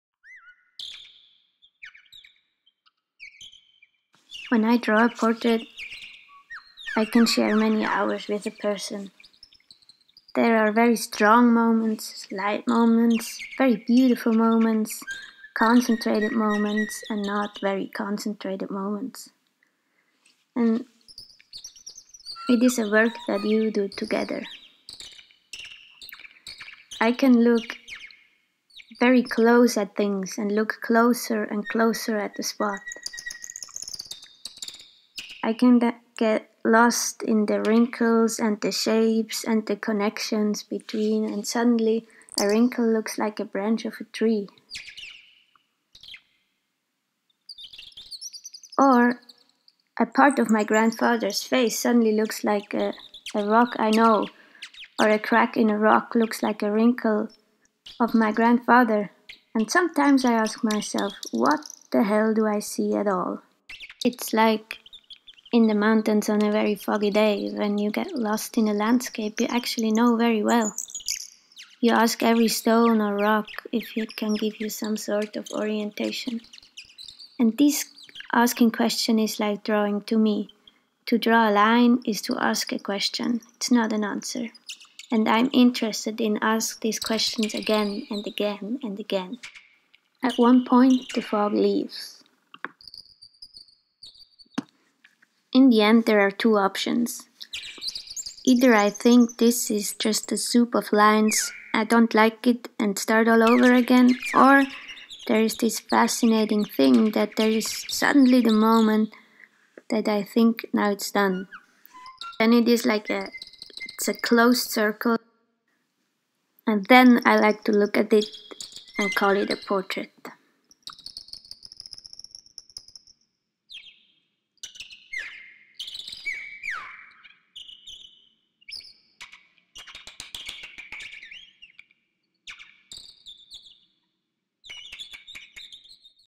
MVELA Film Flavia Somalvico
Flavia Somalvico, terza classificata al Concorso per giovani incisori, racconta come nascono le sue opere: un video poetico sul legame tra natura e arte, osservare e porsi domande.